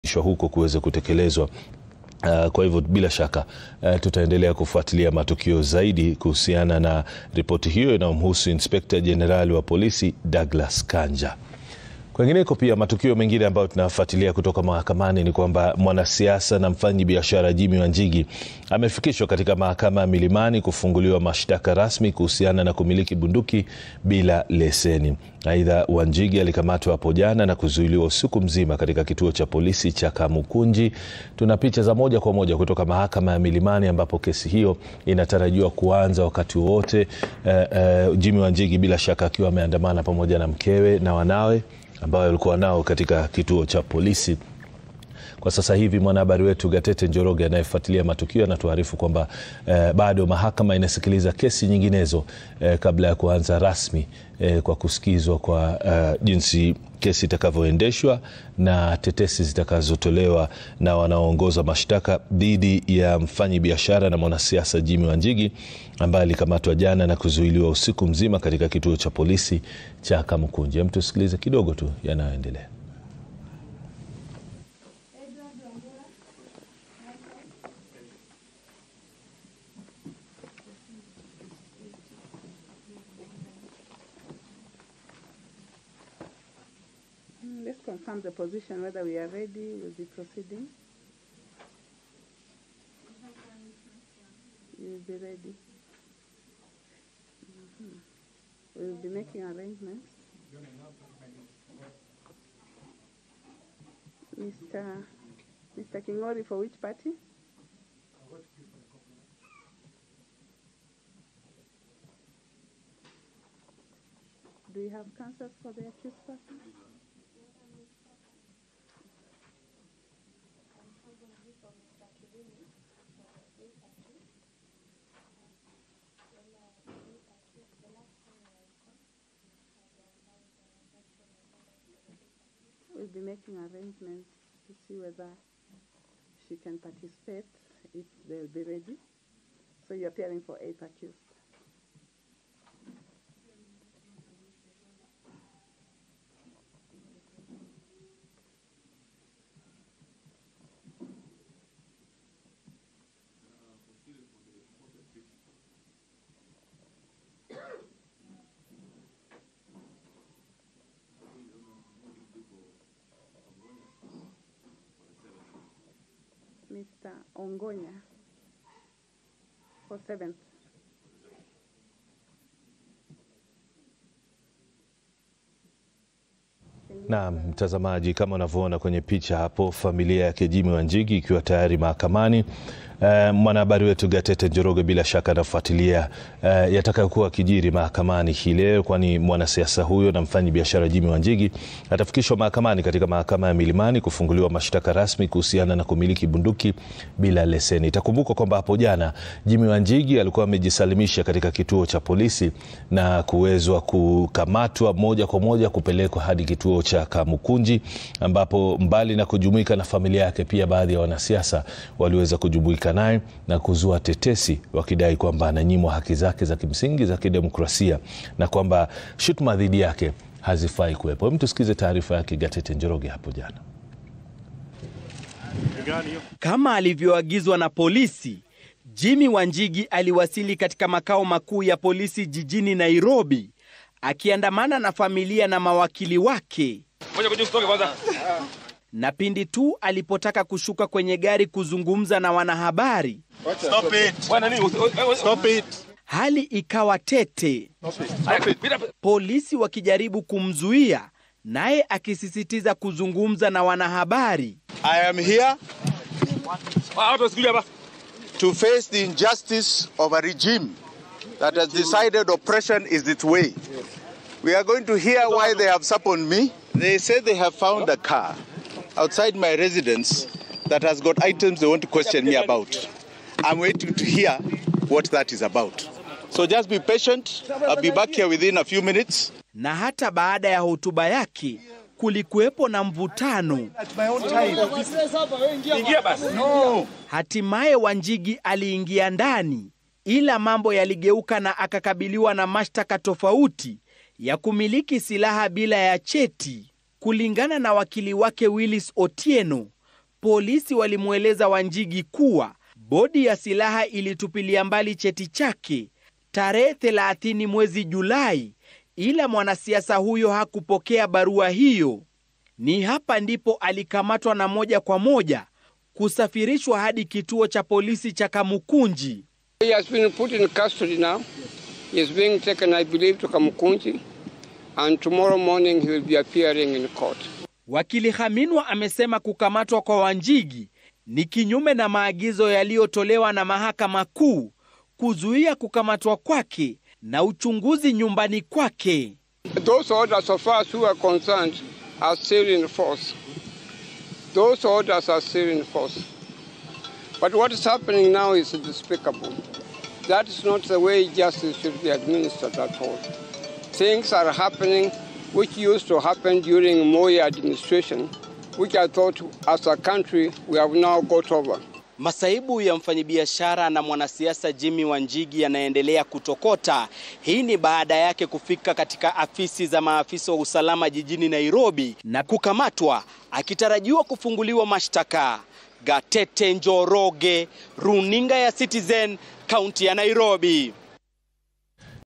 Mfanyabiashara Jimi Wanjigi amewasilishwa katika mahakama ya Milimani
Jimi wanjigi anatuhumiwa kumiliki bunduki bila kibali